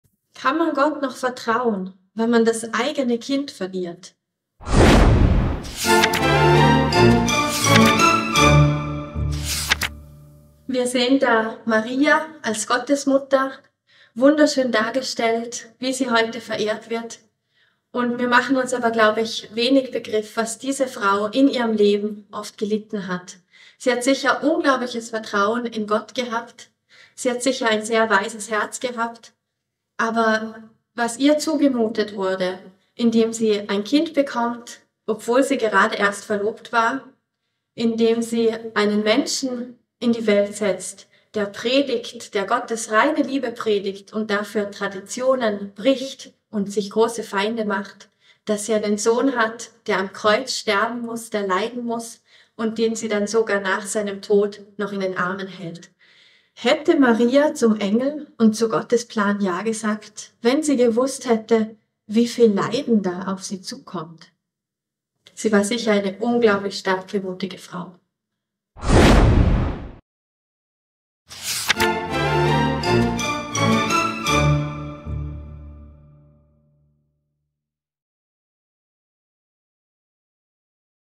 Maria - Frauen in der Bibel
Maria - Die Gottesmutter

Stell dir vor, du bist ein junges Mädchen, gerade mal verlobt, und ein Engel verspricht dir, dass du ein Kind bekommen wirst, ein ganz besonderes – ein Mensch, der allen Menschen Erlösung bringen soll. Und stell dir vor, dein erwachsener Sohn erregt so viel Aufsehen, weil er Gottes reine Liebe predigt, viele Wunder bewirkt und sich durch seine radikale Nächstenliebe auch große Feinde macht. Ganz im Sinne des Muttertags am 14. Mai möchten wir im dritten Teil der Serie "Frauen in der Bibel" einen Blick auf die Gottesmutter Maria werfen. Sie ist der Inbegriff einer idealen Mutterrolle – liebevoll, fromm, Gottes Willen bedingungslos annehmend.

Vielleicht können wir uns aber kaum vorstellen, wie herausfordernd es manchmal gewesen sein muss, die Mutter von Jesus zu sein? Maria wurde von Gott auserwählt, und ihrem Herzen wurde enorm viel zugemutet. Kaum vorstellbar, wie sie selbst gelitten haben muss, als Jesus gequält und ans Kreuz genagelt wurde. Bis nach seinem Tod ist sie bei ihm geblieben, zusammen mit anderen Frauen, die das mindeste, was sie konnten, für den Sterbenden taten: ihn nicht alleine lassen, und sein Leid aus nächster Nähe aushalten. Und als er vom Kreuz abgenommen wurde, hielt sie ihn noch eine Weile in ihren Armen. Wir wissen nicht, ob sie in dieser Stunde geahnt hat, dass er den Tod überwinden wird.

Wenn jede Mama der Welt die Reise ins Ungewisse herausfordernd findet, auf welcher sie ihre Kinder begleiten darf – so war es für Maria vielleicht auch die intensivste Erfahrung ihres Lebens.

präsentiert von
Aglaia Poscher-Mika
Frauenreferentin der Katholischen Kirche Vorarlberg
Herzlichen Dank an das Bibellabor (bibellabor.at) für die inhaltliche Unterstützung.